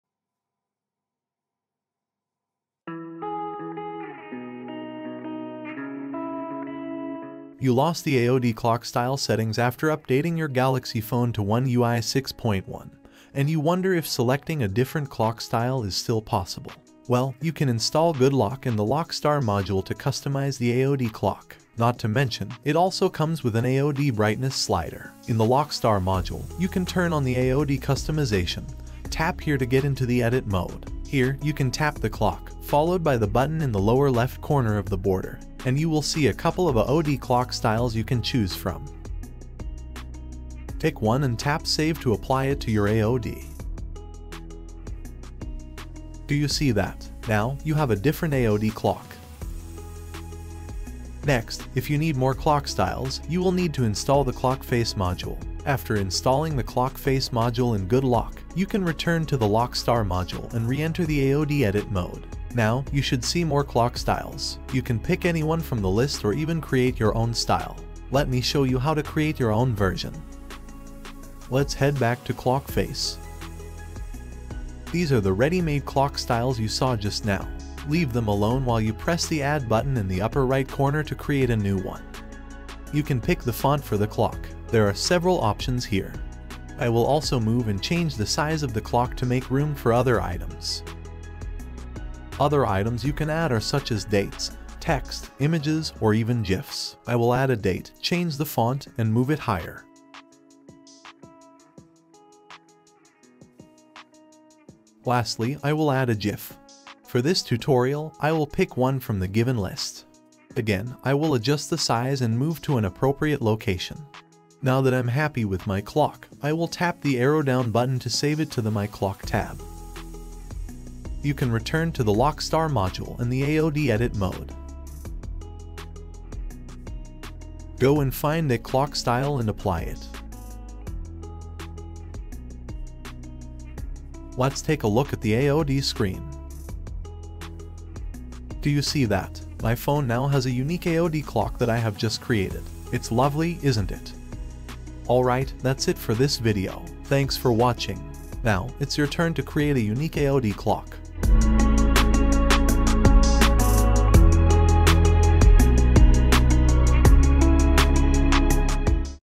How to get more AOD clock styles in One UI 6.1?
In this video, you will find out the ways to get many more AOD clock styles in Samsung One UI 6.1?

Other links:
Samsung Galaxy phone Q&A (English)